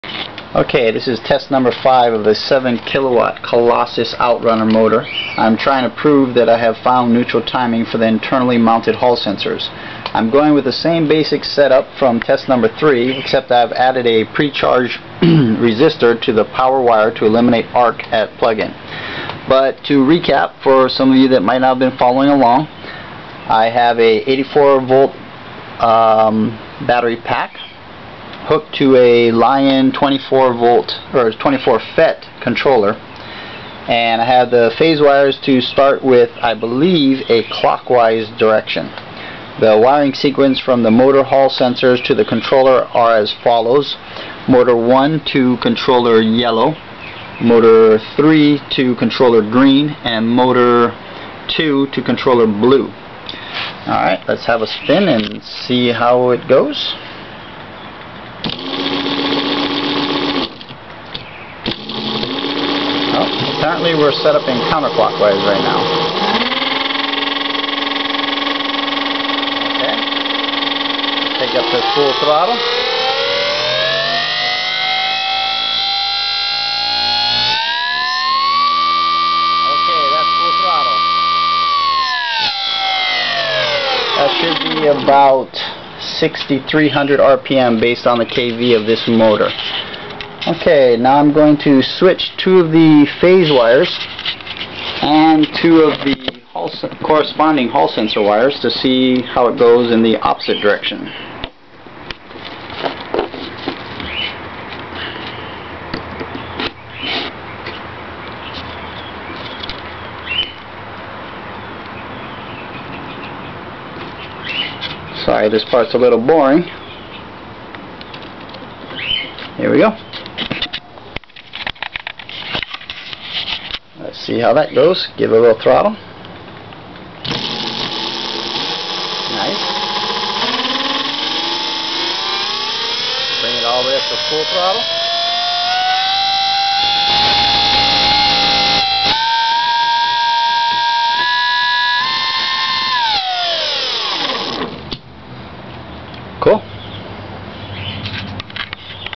7kw Colossus Test 5: Finding Neutral timing
This is the 5th bench test with the 7kw 75kv Colossus Brushless Outrunner Motor.  84 volt battery pack with Lyen 24 fet controller.  I had just installed hall sensors and I'm trying to determine if I have them timed for neutral or not.